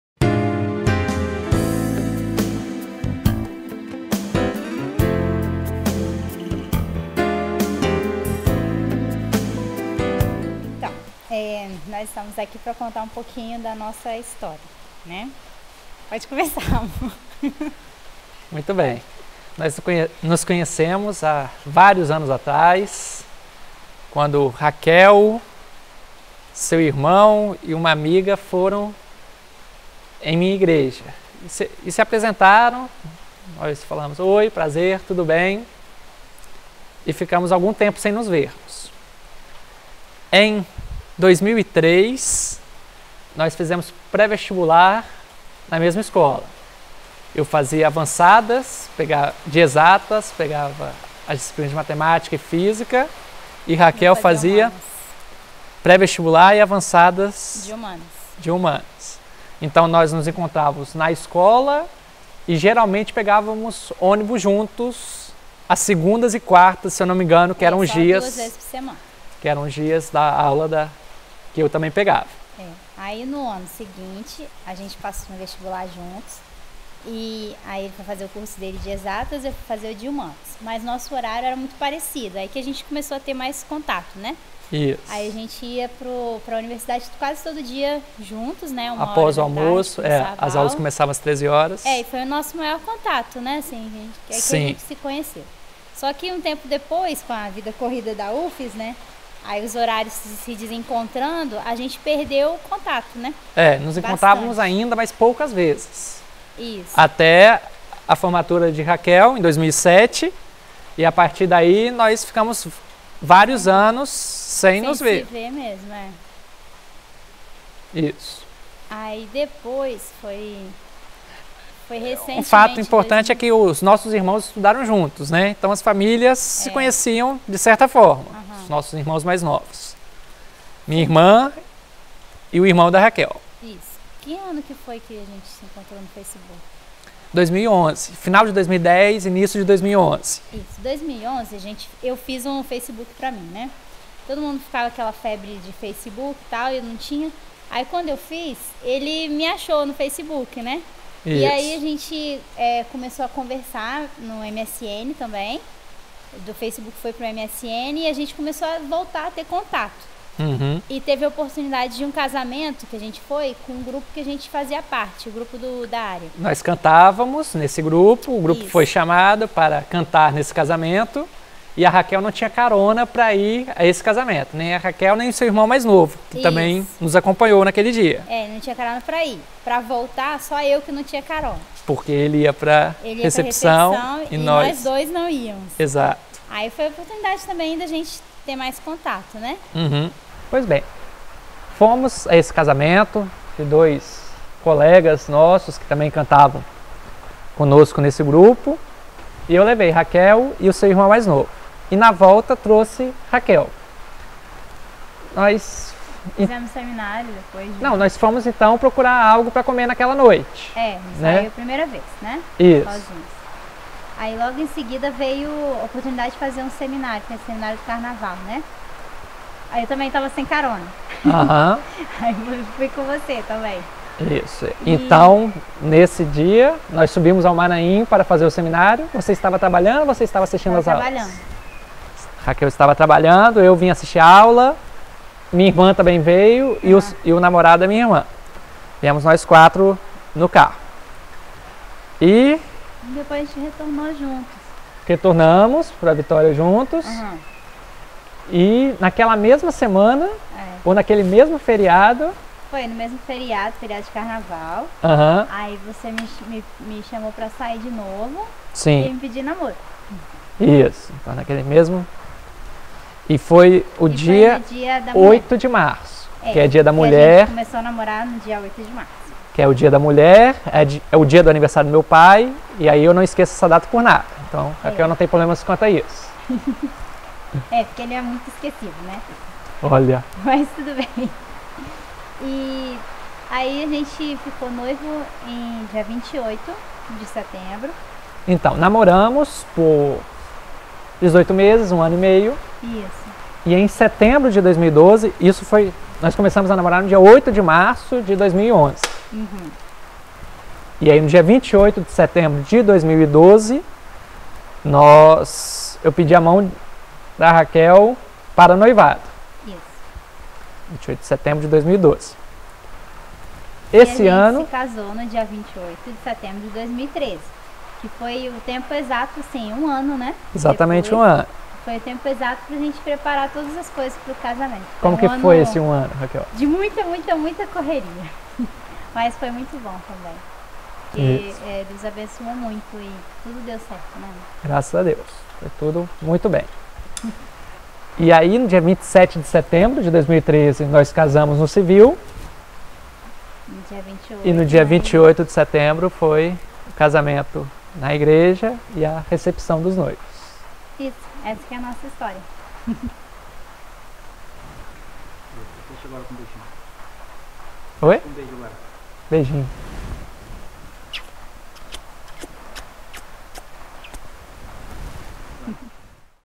Love Story
Conheçam um pouco da história desse lindo casal
Erick & Raquel